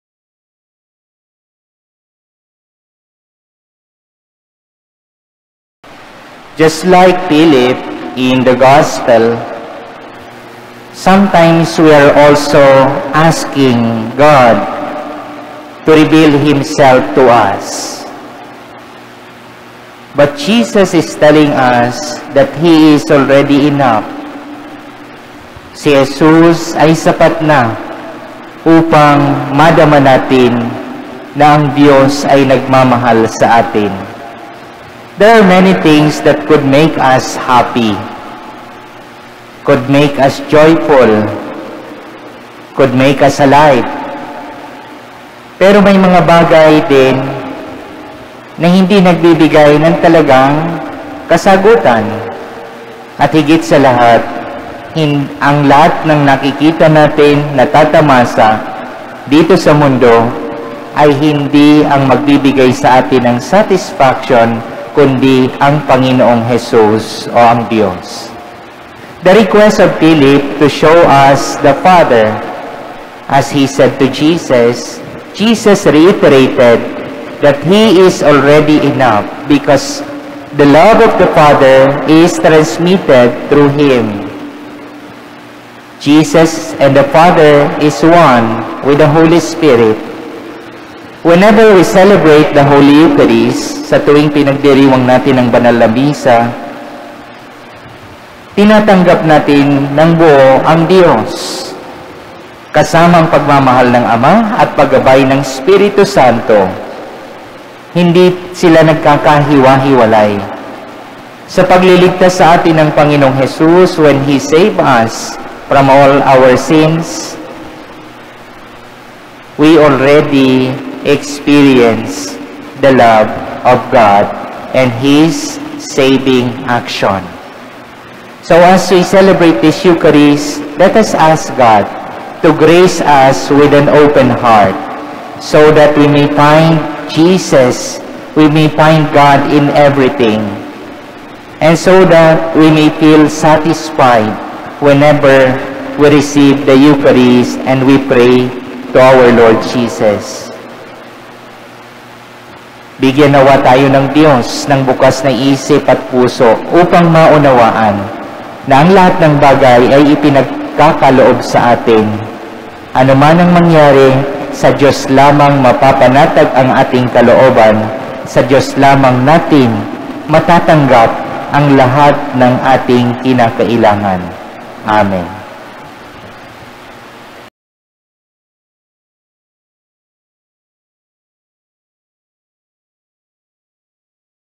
HOMILY FOR THE DAY | Feast of Saints Philip and James, Apostles
May 3, 2023 | 7:30 AM

Presided by: Rev. Fr. Rey Hector A. Paglinawan, Parochial Vicar of Cubao Cathedral

The Gospel According to Saint John 14:6-14